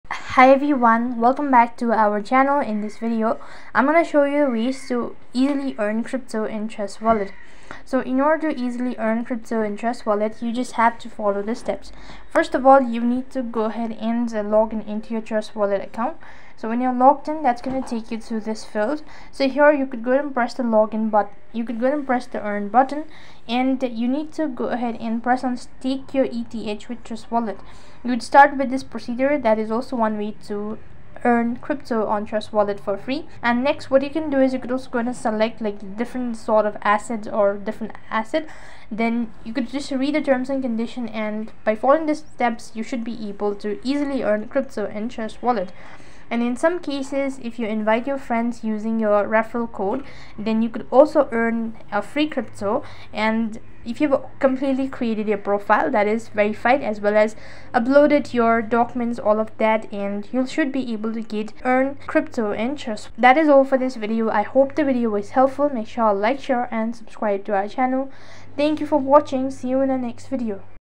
How To Easily Earn Crypto In Trust Wallet [Staking Guide] !
This Video Is based on How you can easily earn crypto in Trust Wallet.

Chapters / Timestamps
0:00 Introd
0:19 Easily Earn Crypto In Trust Wallet
1:03 Outro